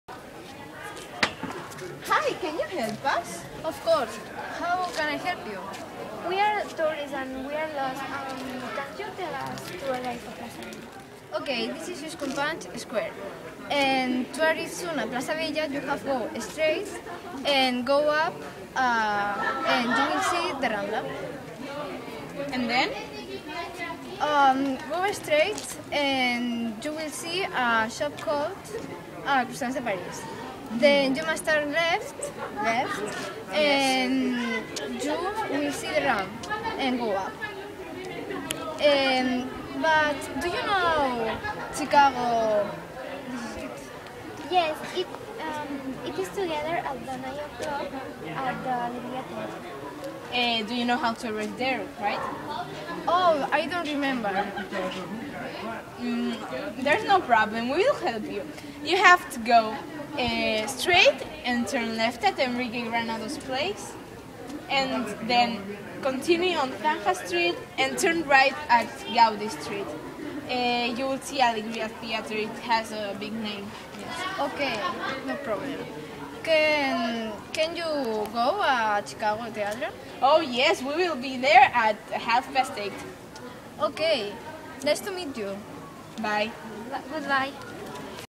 We are lost! Alba, Sara & Alexandra
Two students are lost in Terrassa and ask a local for help.